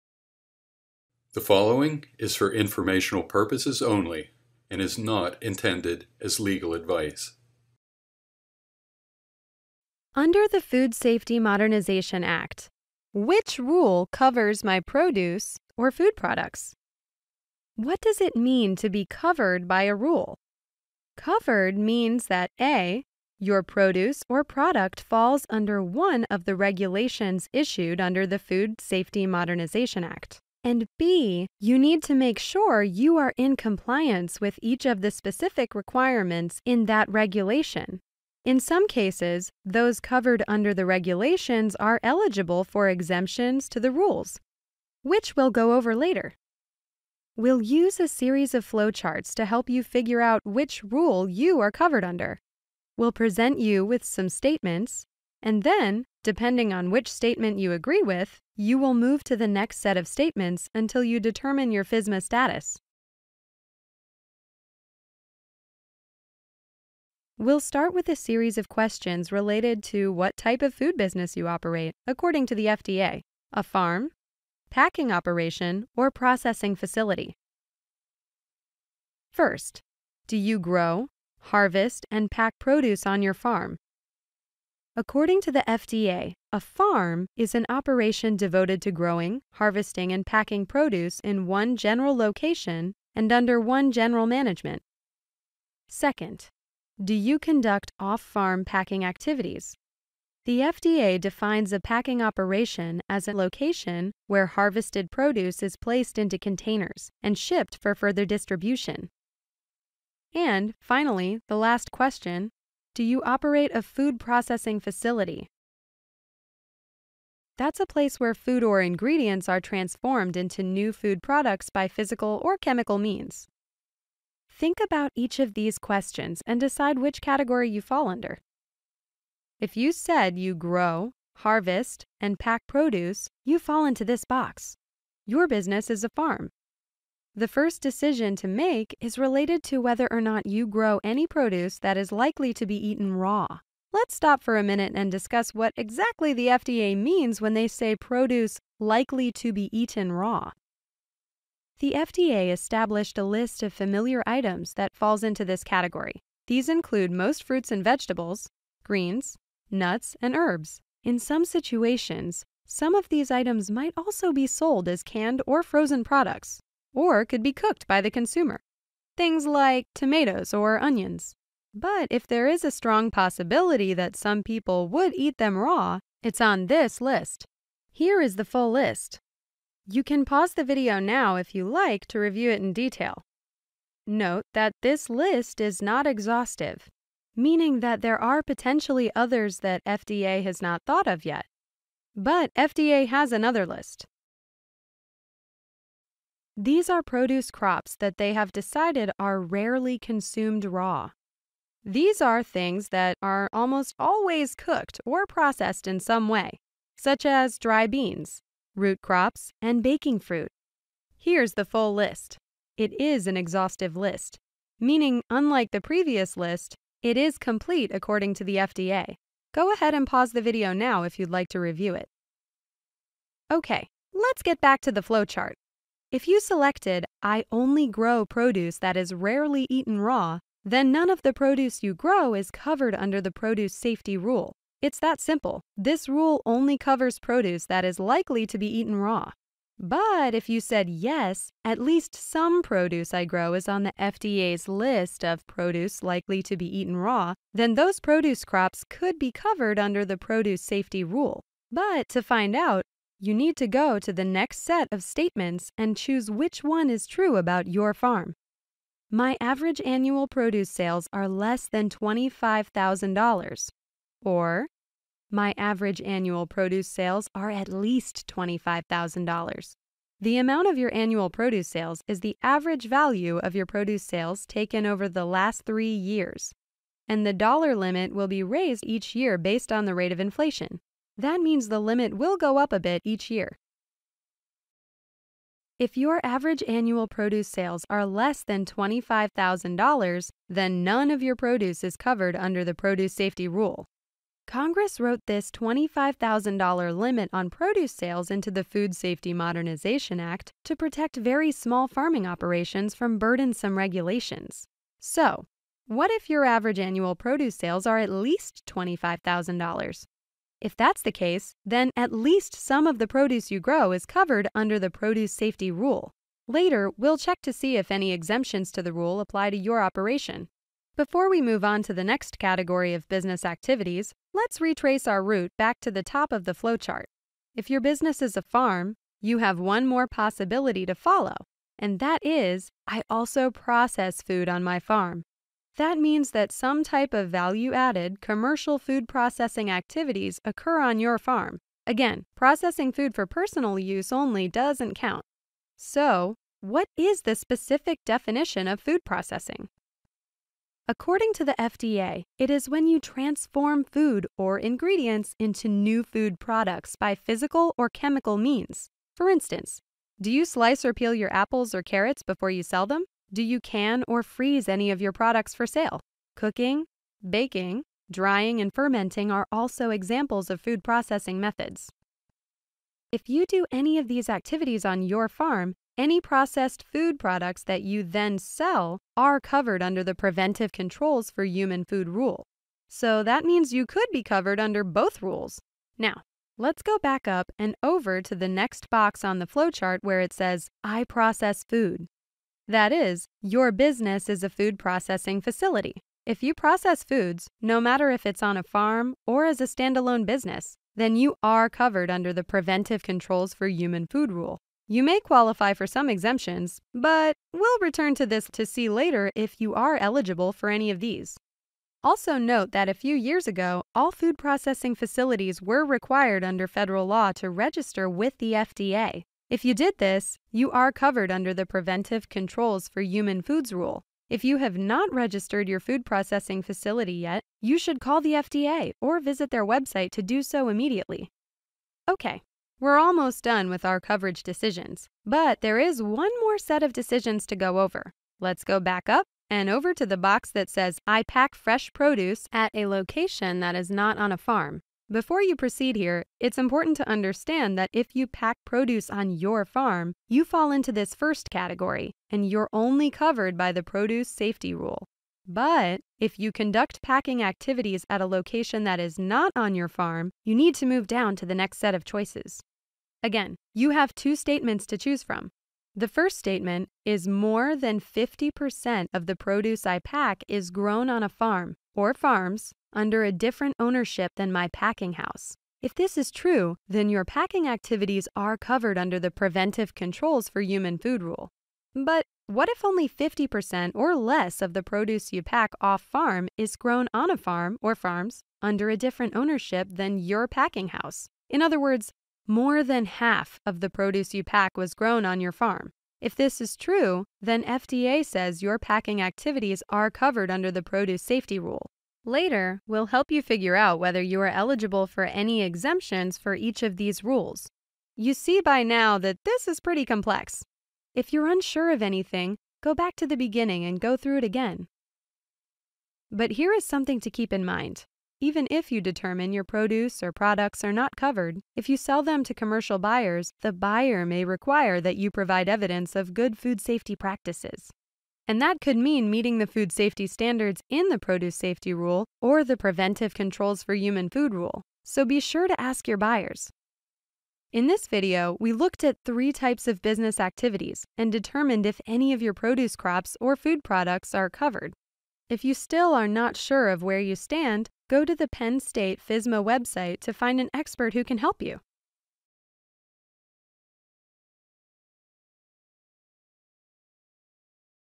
FSMA_Human Food Rules Playlist_2: Which Rule Covers My Produce or Human Food Products?
This video focuses on the Produce Safety and the Preventive Controls for Human Foods rules under the Food Safety Modernization Act, and will help you determine whether your produce crops or food products are covered.